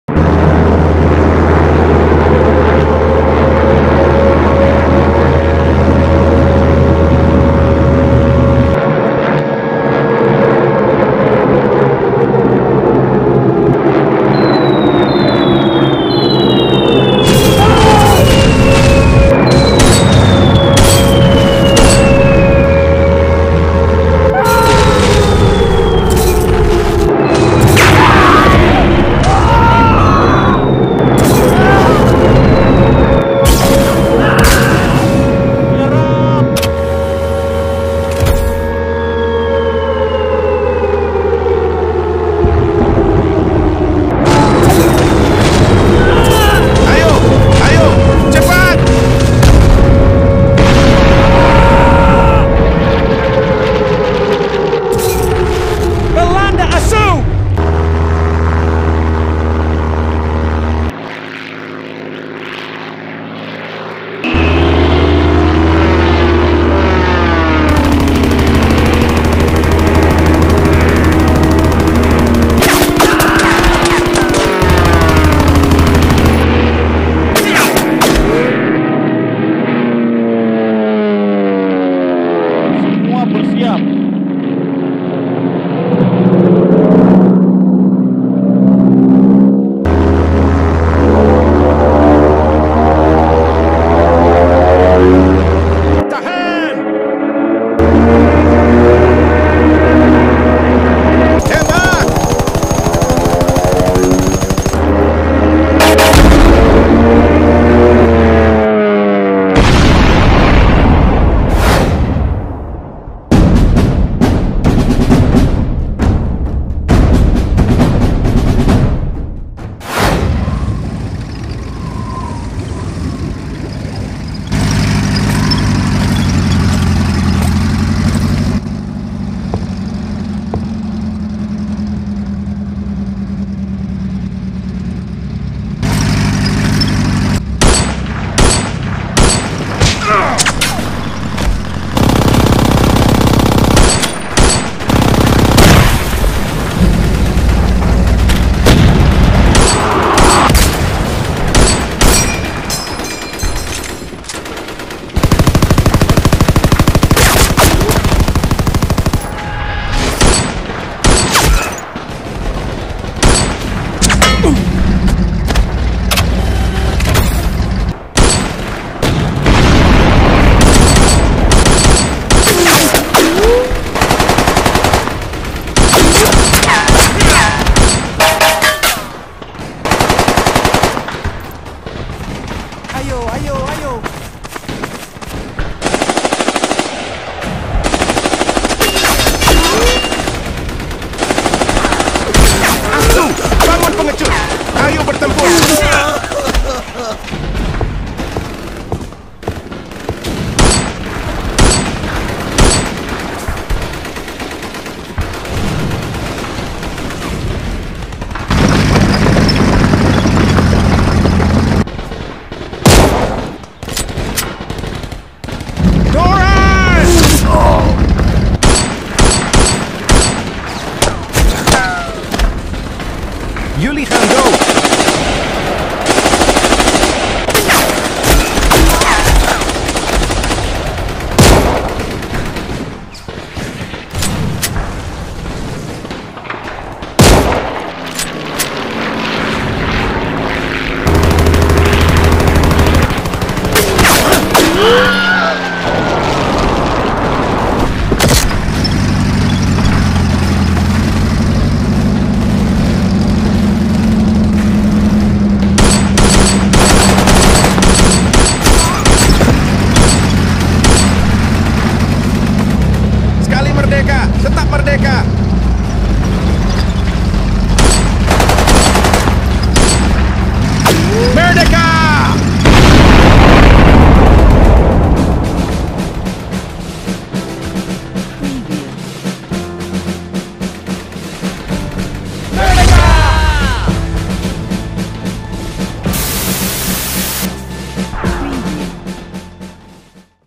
🇮🇩🇳🇱1947 : Dutch Military Aggression | Stickman War | War Animation
Happy 79th independence day for Indonesia 🇮🇩🇮🇩🇮🇩🇮🇩
i dedicated this animation to the fallen Indonesian freedom fighter and whole Indonesian.